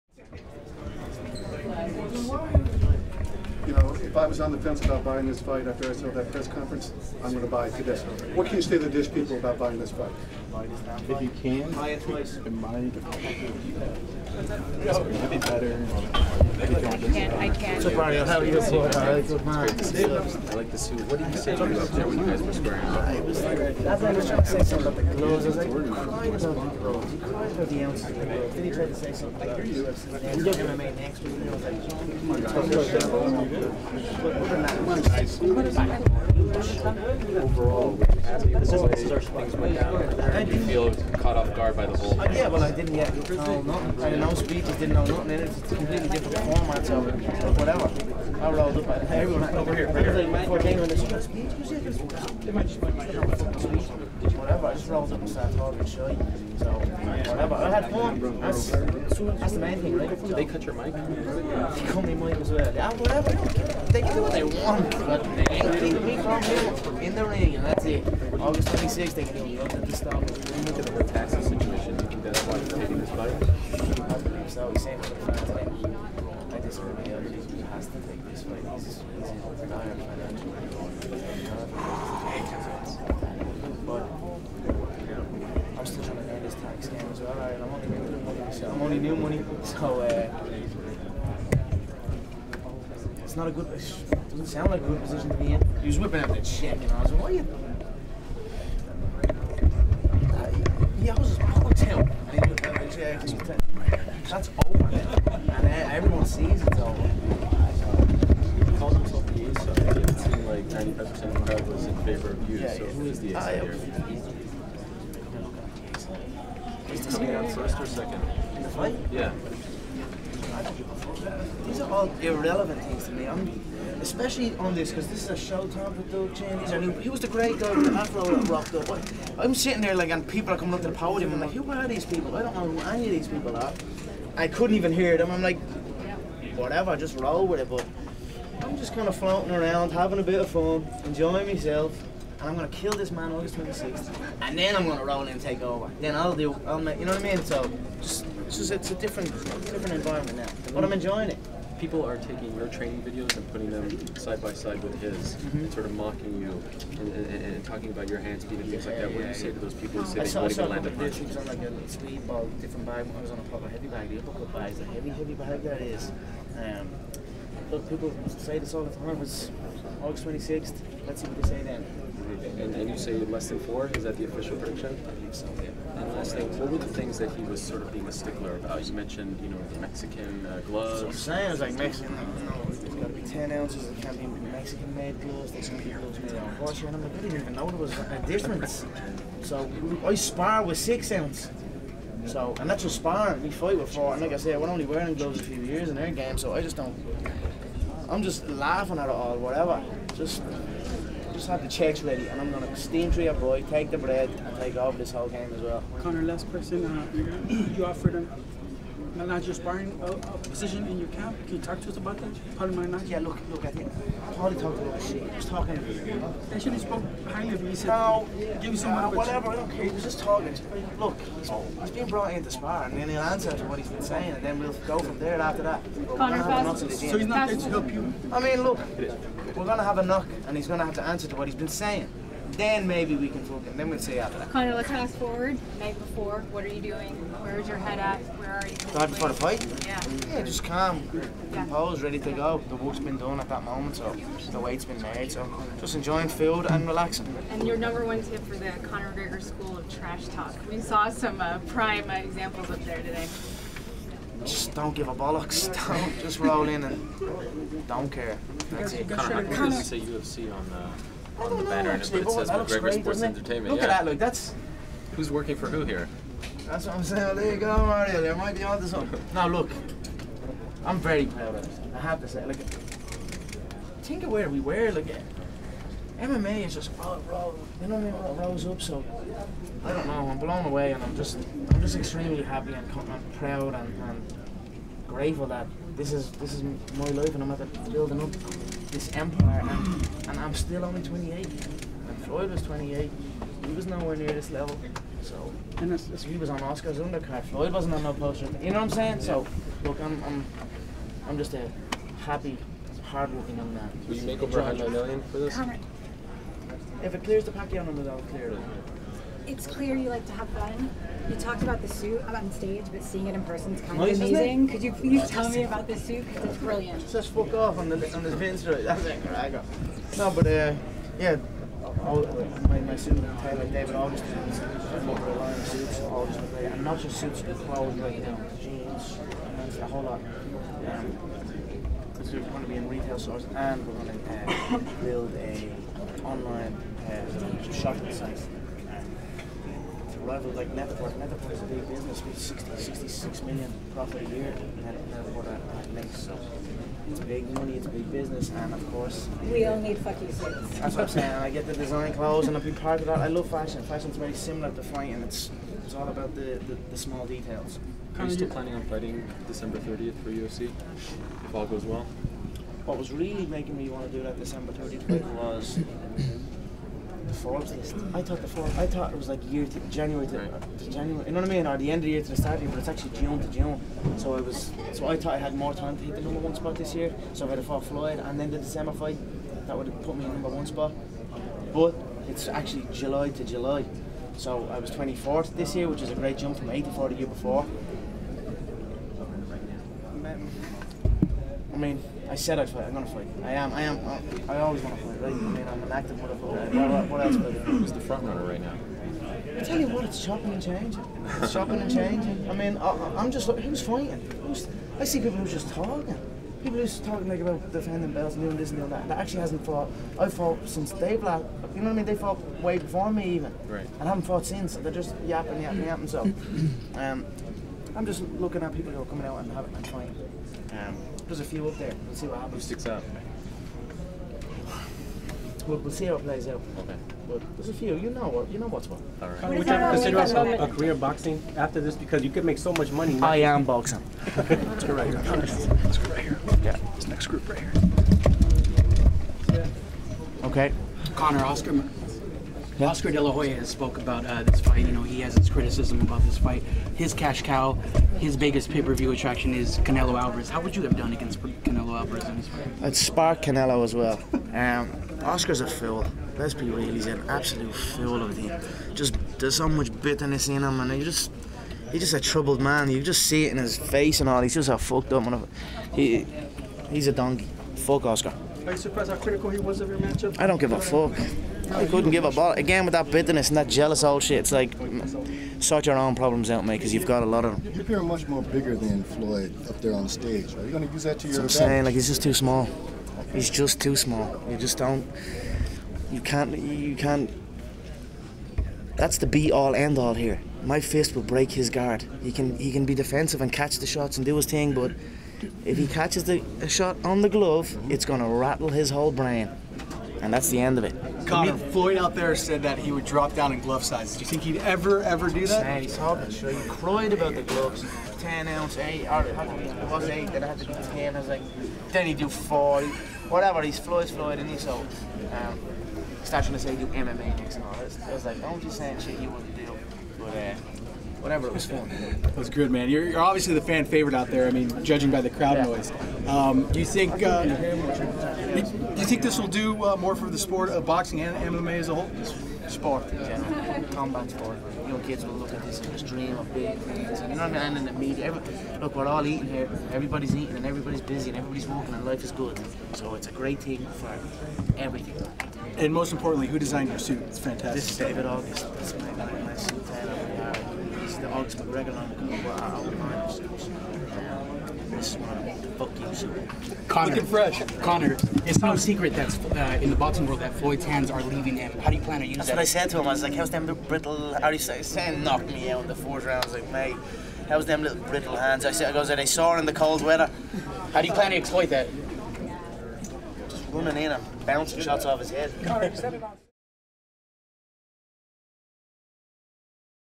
Conor McGregor Taunts Floyd Mayweather: “How’s Those Little, Brittle Hands?” MayMacWorldTour LA
MMA H.E.A.T. caught up with UFC Lightweight Champion Conor McGregor backstage at the Staples Center in Los Angeles, California.  Conor shares his thoughts on the 1st press conference and facing off with Floyd Mayweather for the first time, his predictions for their August 26th fight + more!